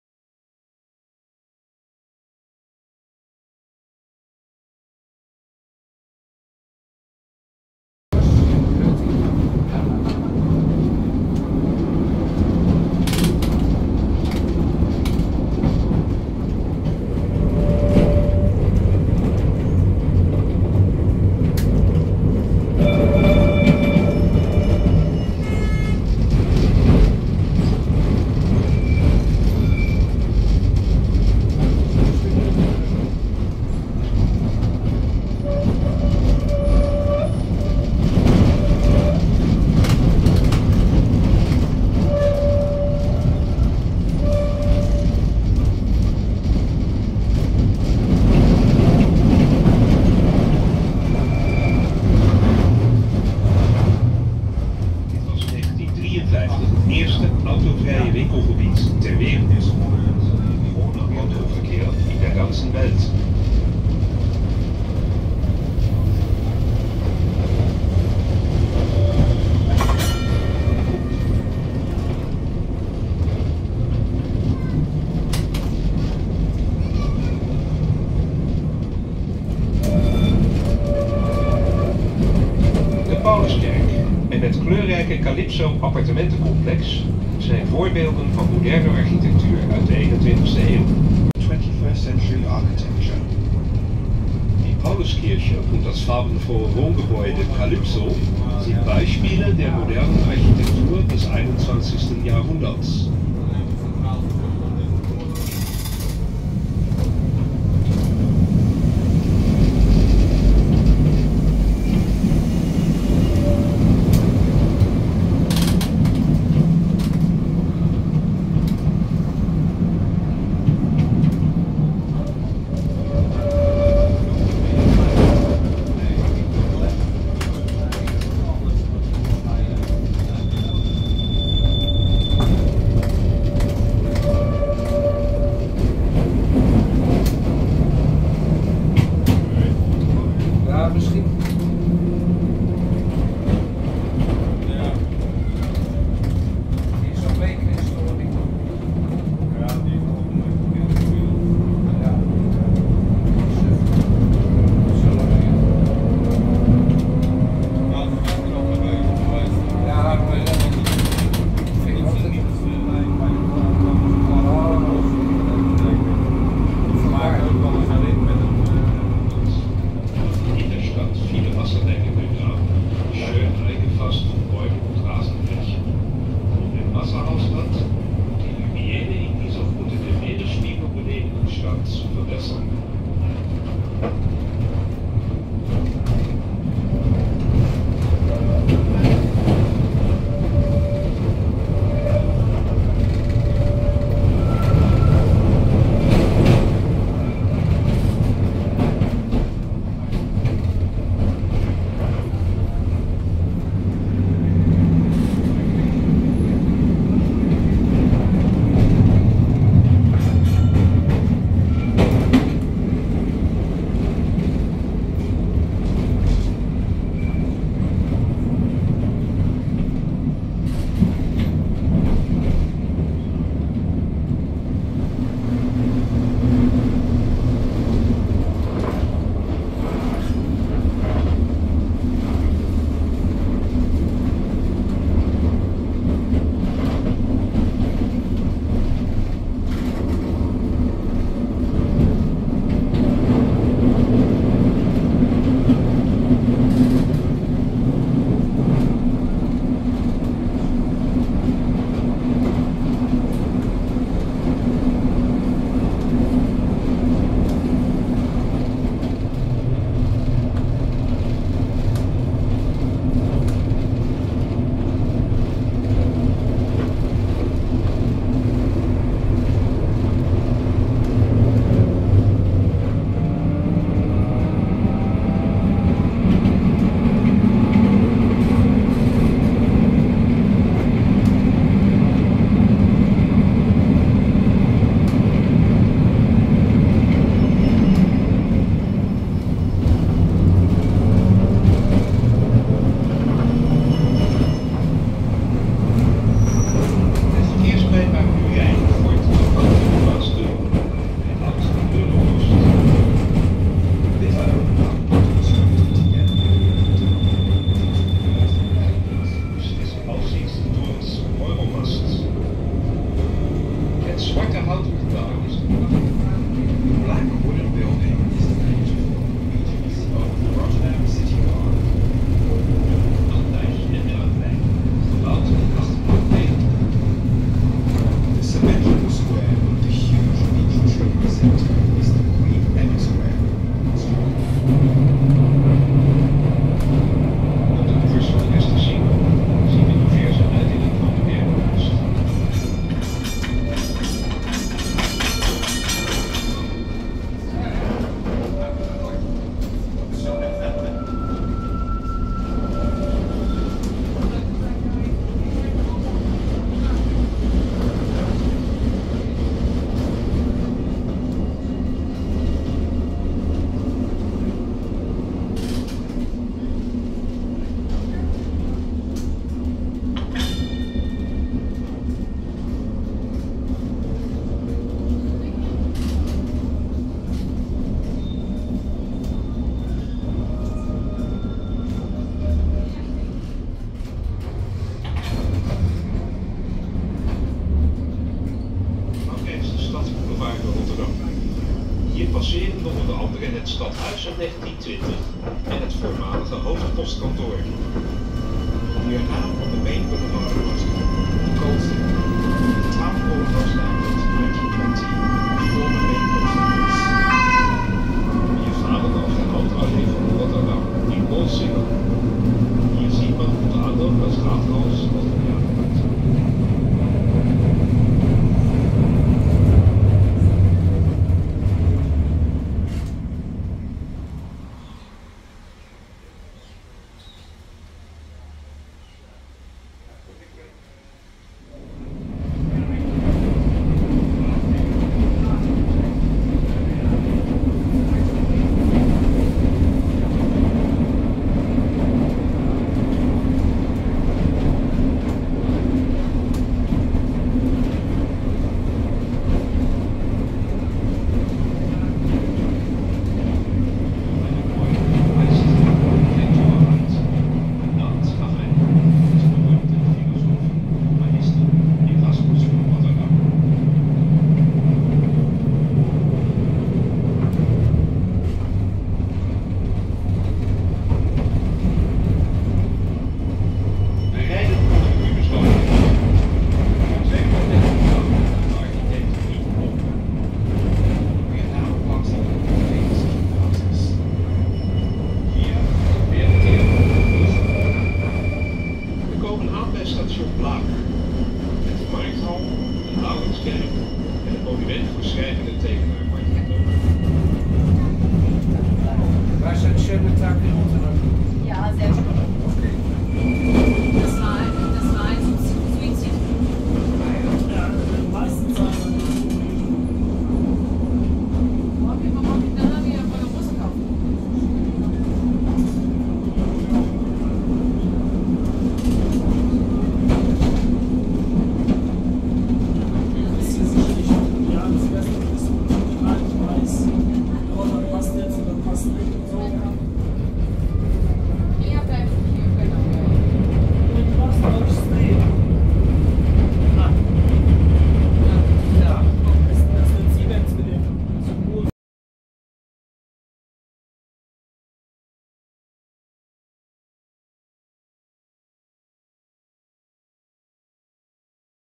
Rondrit door Rotterdam met historische tram Lijn 10
Impressie van een leuke rondrit door Rotterdam met een fraaie historische tram.

Meer info?  Kijk op of